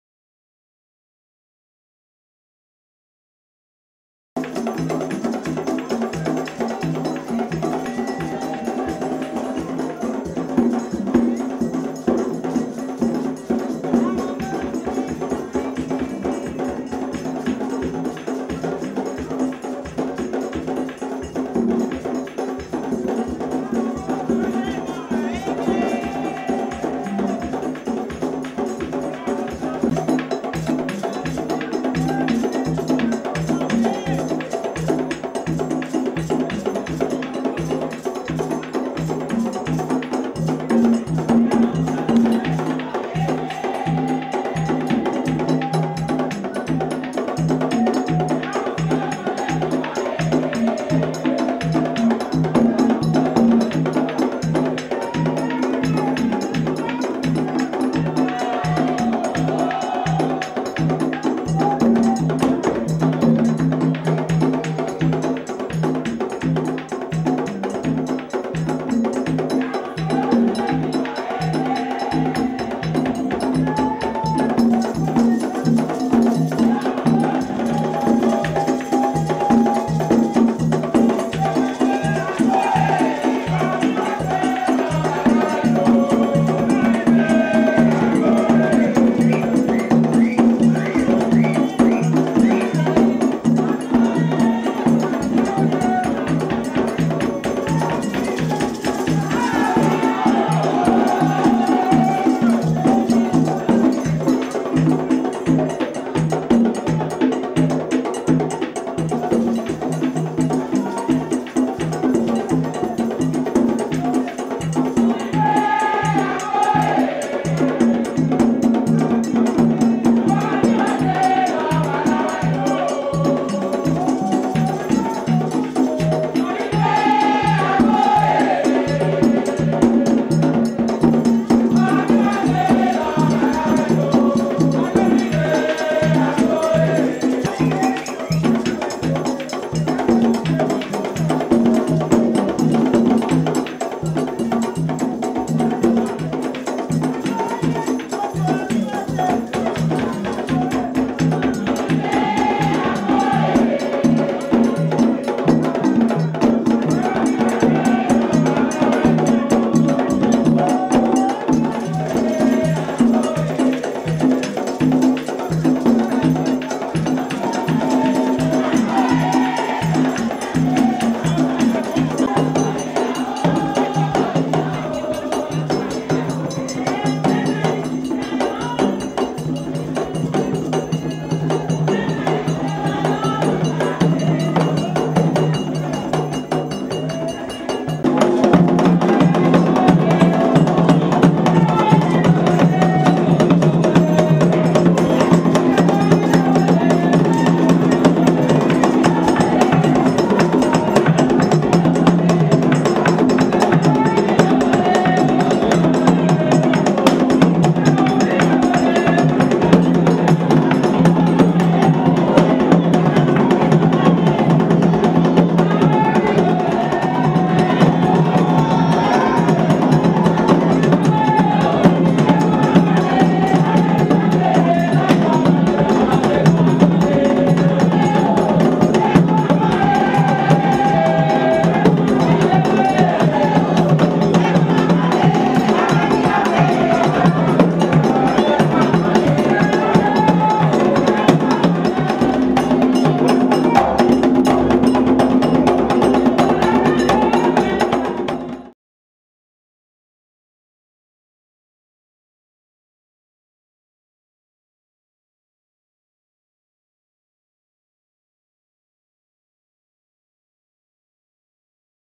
dantor's ceremony in montreal.
ezili dantor,ezili gerouge,ezili mapiange,ezili baliane,ezili laflambeau,ezili kenoue,their all celebrated the same time most of the time,dantors yearly ceremony in montreal canada by labelle deesse dereale vodou temple.